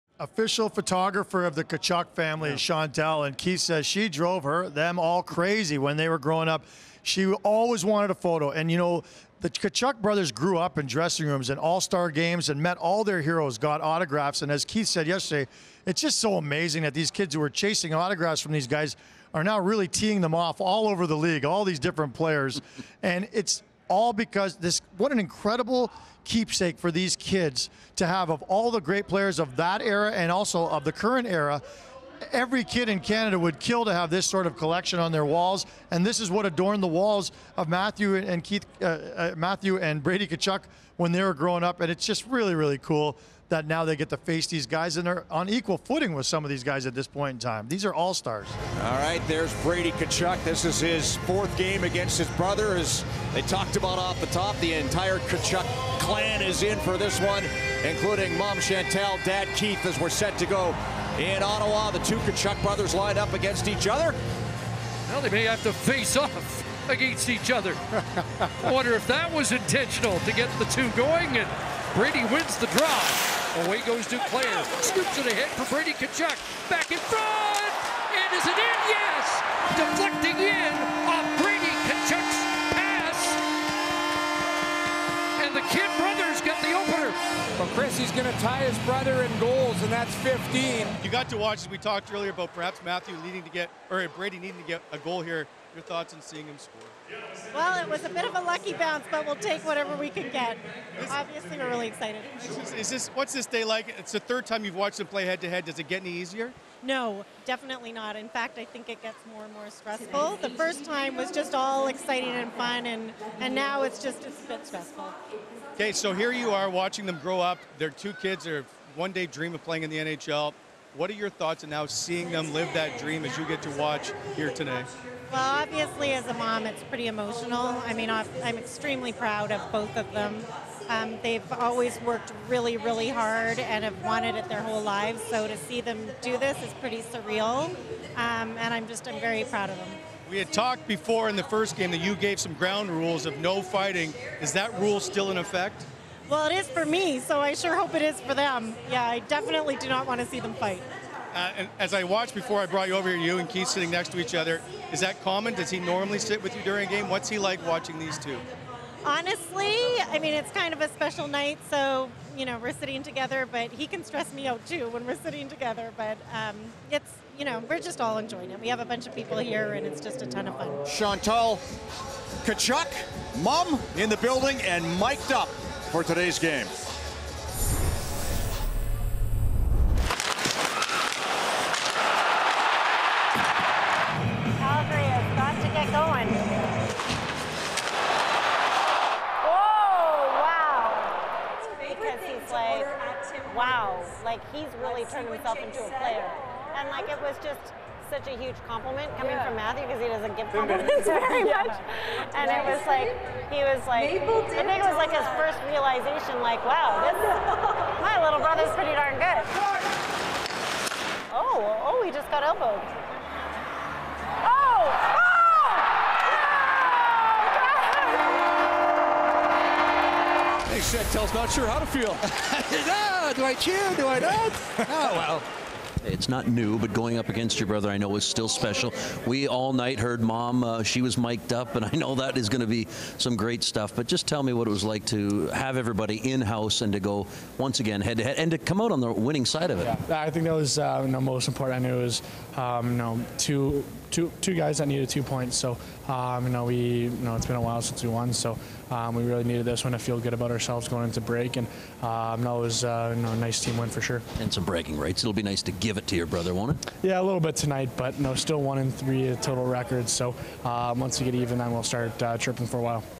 Brady takes home 1st victory over Matthew as Tkachuk family watches on
Brothers Brady and Matthew Tkachuk squared off again, with Brady scoring and leading the Ottawa Senators to a 5-2 over the Calgary Flames with the entire Tkachuk family looking on

Feeling social?